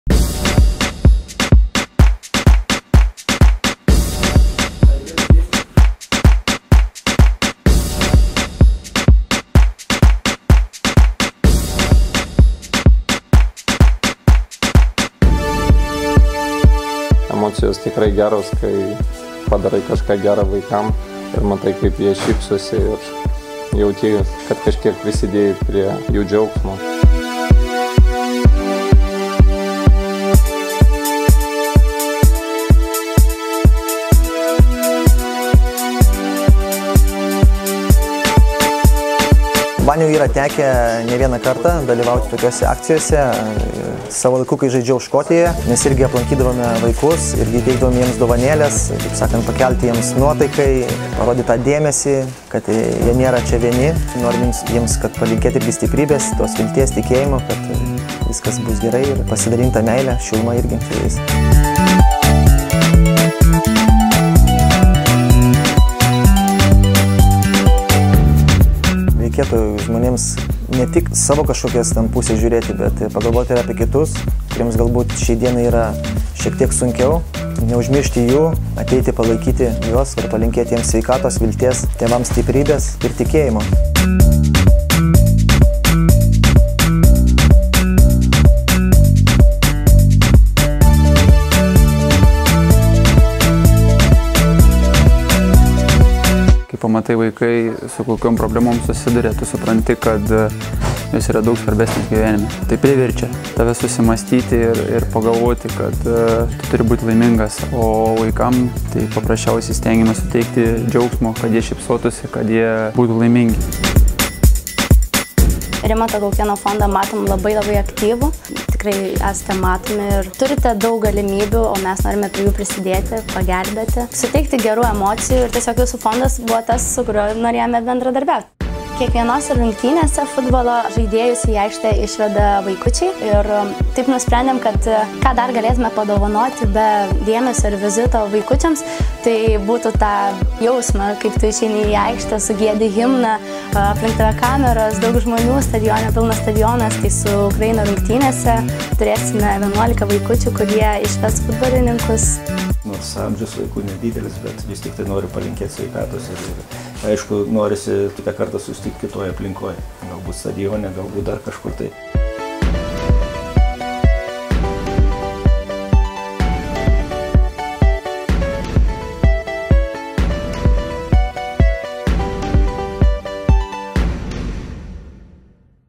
Lietuvos rinktinės futbolininkai aplankė mažuosius pacientus
Vakar diena Santaros ligoninės vaikų onkohematologijos skyriuje buvo nuspalvinta vaikų šypsenomis, nes čia mažuosius pacientus aplankė Lietuvos futbolas rinktinės žaidėjai Mantas Kuklys, Saulius Mikoliunas, Fedor Černych ir treneris Valdas Urbonas, kurie ne tik dovanojo vaikams savo dėmesį bet ir įteikė specialius futbolo sirgaliaus marškinėlius.